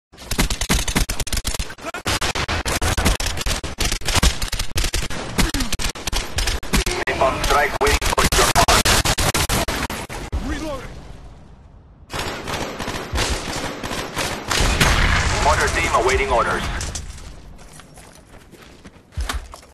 I can has god mode MOAR?! - TDM on Villa
Basically a temporary god mode type of clip. A good kill, then an enemy fails to kill me with a SPAS-12, then both opposing players fail to kill me.. and I kill both of them with an RPG without killing myself! Absolute god mode!!!